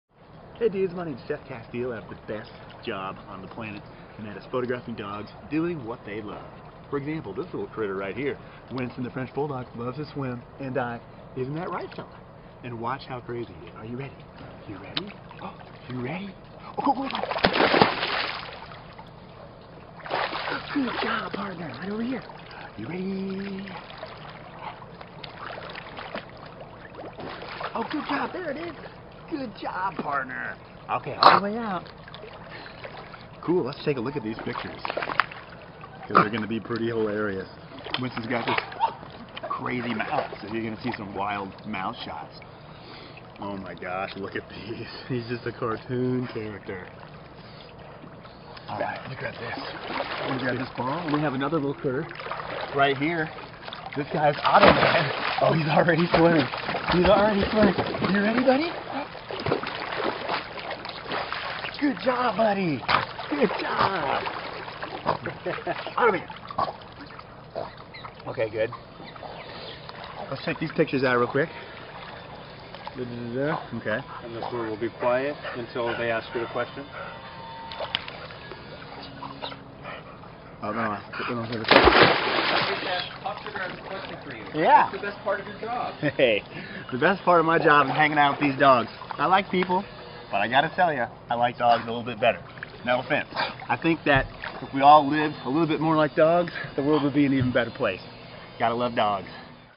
Seth Casteel of "Finding Fido" -- Live!
New York Times bestselling author and photographer Seth Casteel, the host of  "Finding Fido," coming soon from ZLiving and PopSugar, lets you inside his process for capturing his world-famous underwater dog portraits.

In "Finding Fido", Seth helps prospective dog owners rescue the perfect pooch to fit their lifestyle. PopSugar's "Finding Fido: The Dog Who… " tells the stories behind the special unions between dogs and their owner as they prepare to meet Seth, who will help them capture a one-of--a-kind image.  Discover the emotional story of what brought them together in the first place, how both lives were changed forever by the relationship, and how they continue to help inspire each other.